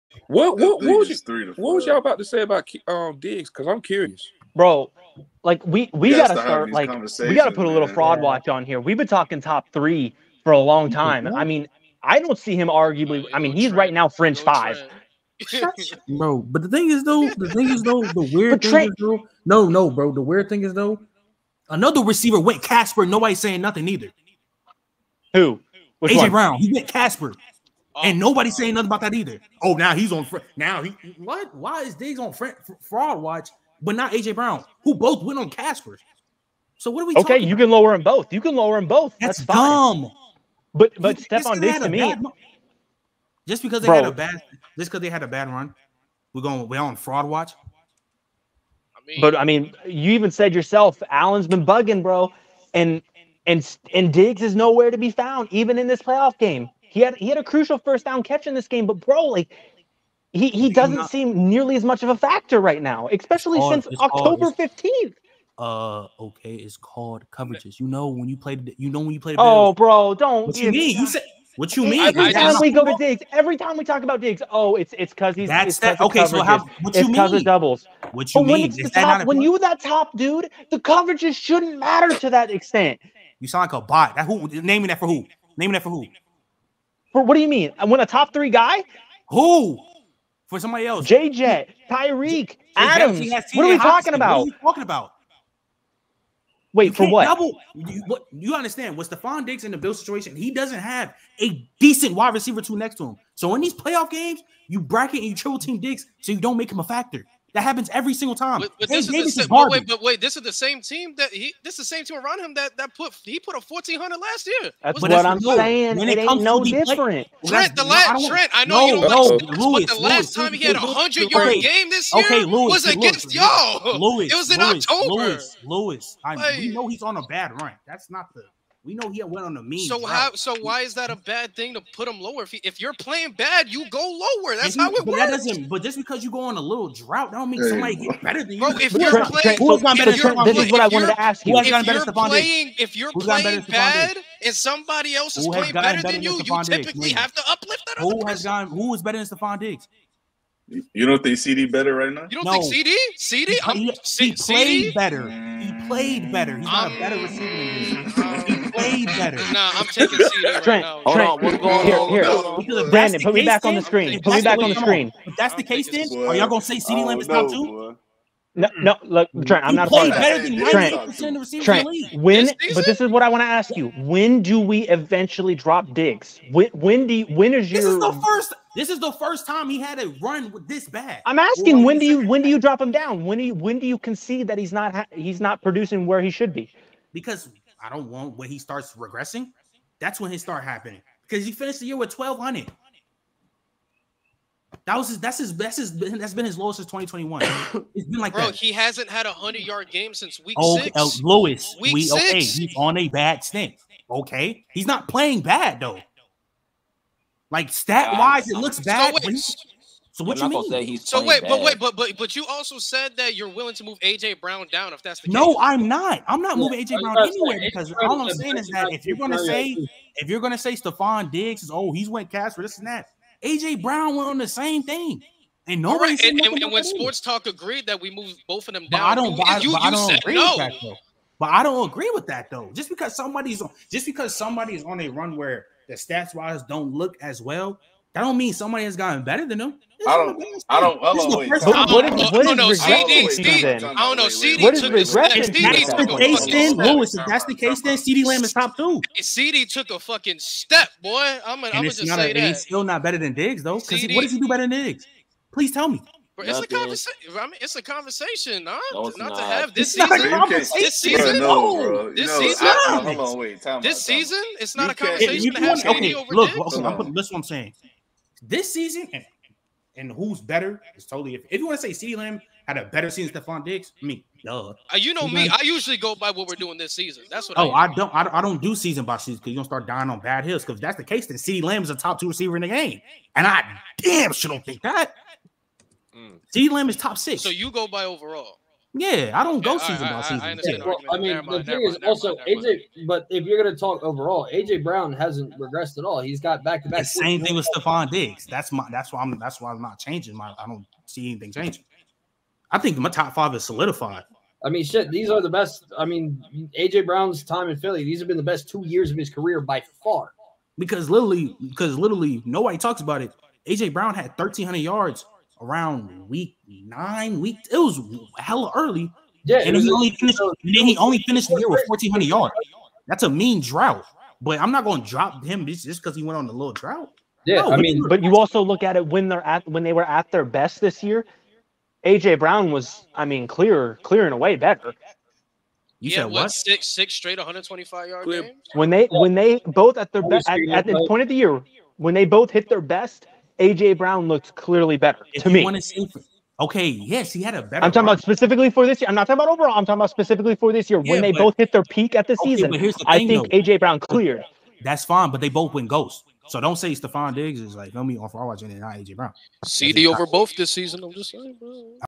Is Stefon Diggs On Fraud Watch?
On The Throwdown Podcast, SportsTalk Said That Stefon Diggs Is on Fraud Watch and It Turned into A Huge Debate.